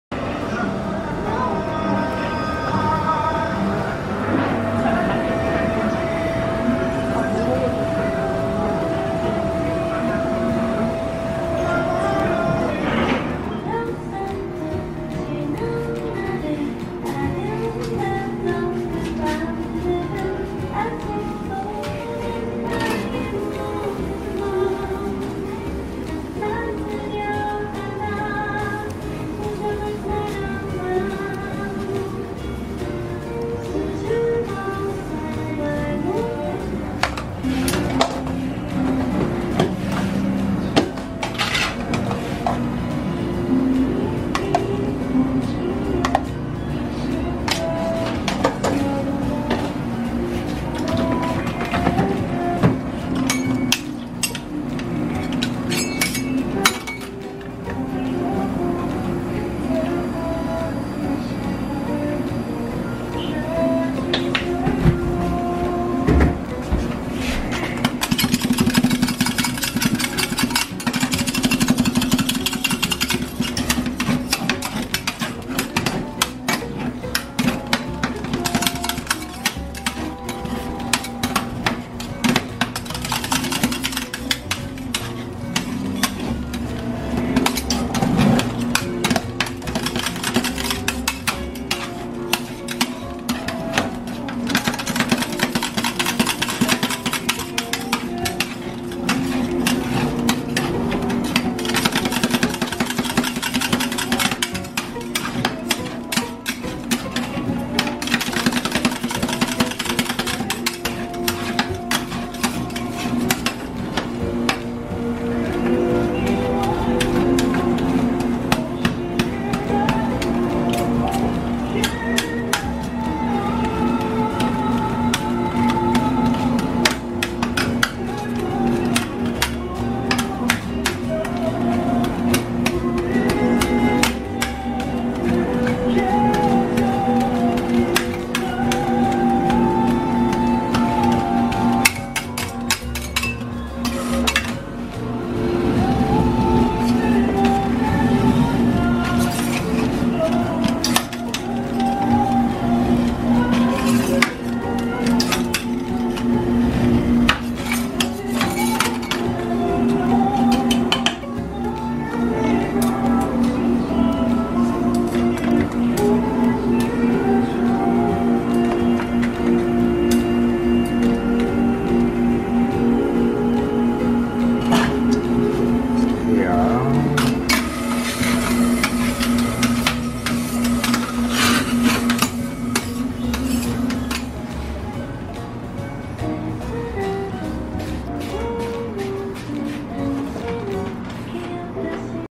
딸기 바사삭 철판아이스크림 / 악마의 레시피 / Strawberry Ice Cream Roll / Korean Street Food
딸기 바사삭 철판아이스크림 / Strawberry Ice Cream Roll
Korean Street Food / 대전 은행동 맛집, 악마의 레시피

[악마의레시피] 철판 아이스크림
주소 : 대전 중구 대종로488번길 45  (지번)은행동 45-12 1층